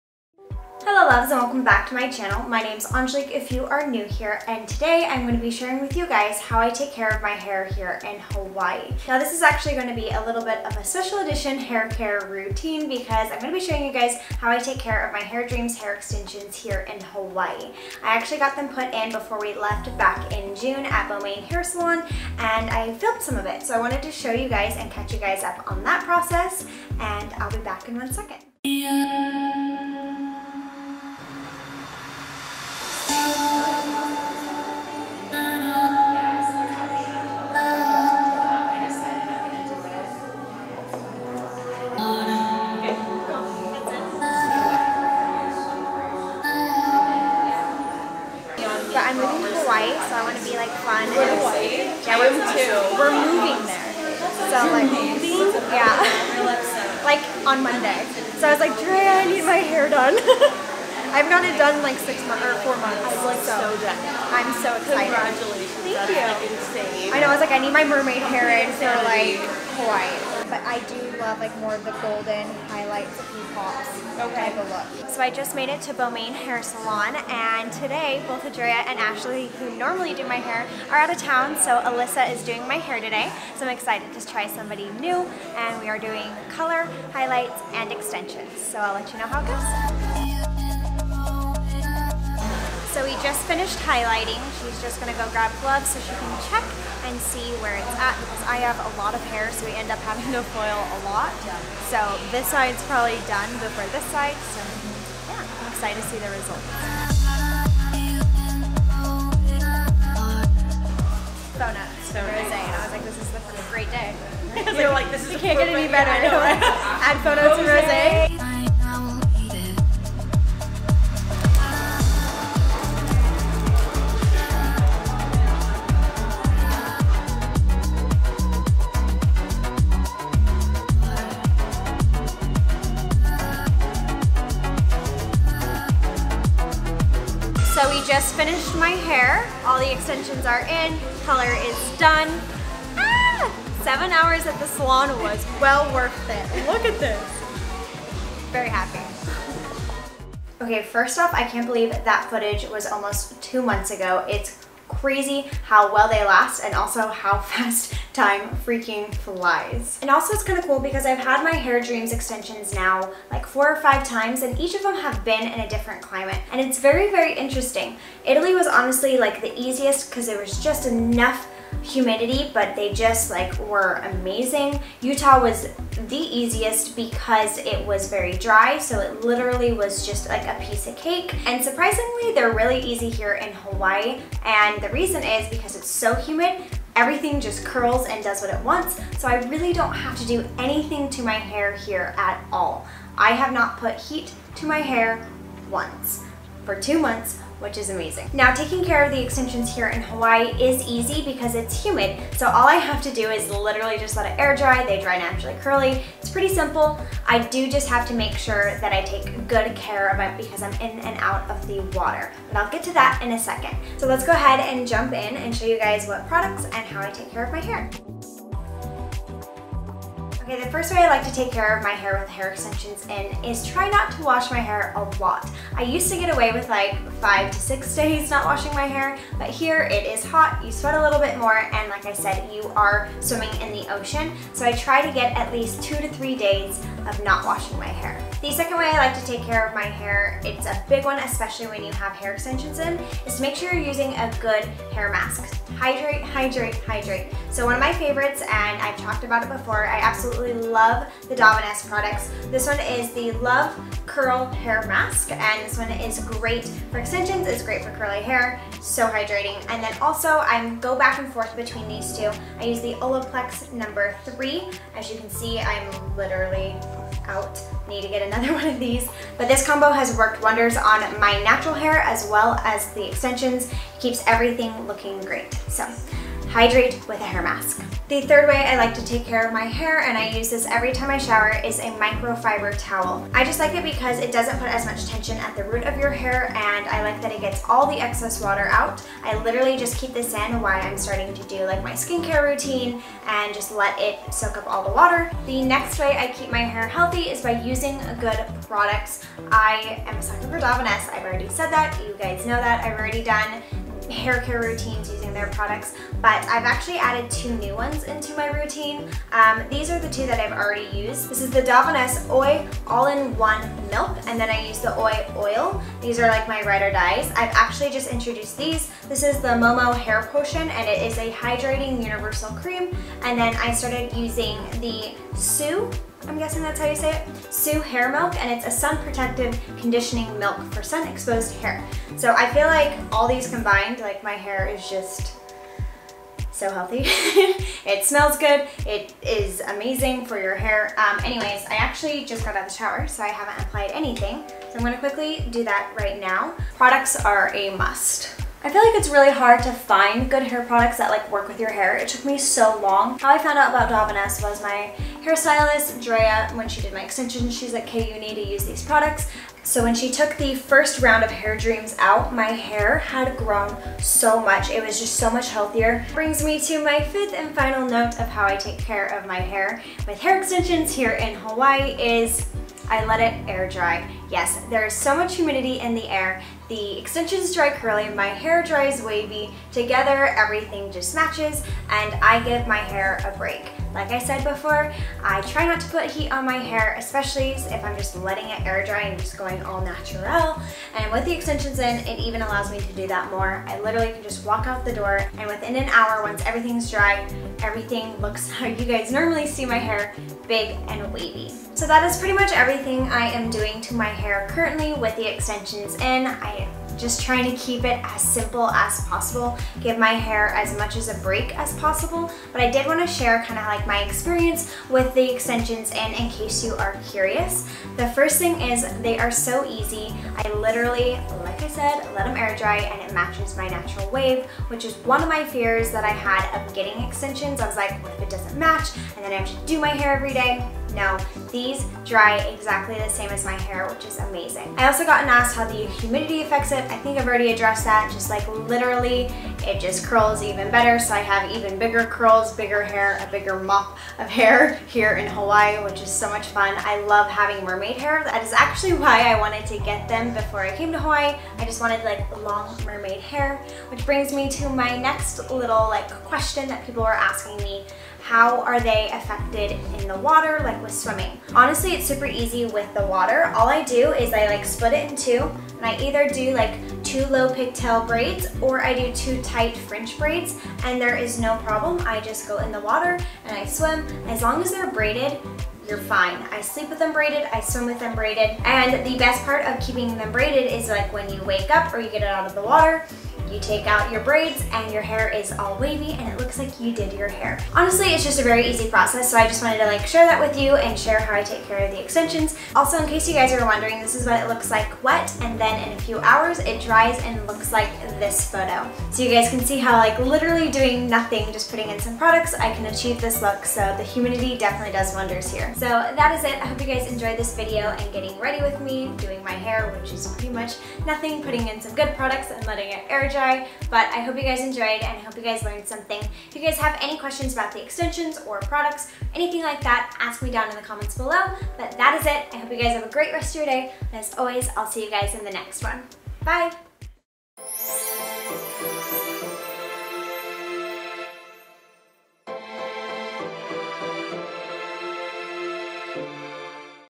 My Hawaii Haircare Routine ft. HAIRDREAMS Extensions
YAY! Finally sharing my Hawaii HairCareRoutine. I currently have HairdreamsExtensions so I show you what products I use and how I take care of my hair! I hope you enjoy! :)

Mailing Address:
Angelique Cooper
111 Hekili St, Ste. A #602
Kailua, HI 96734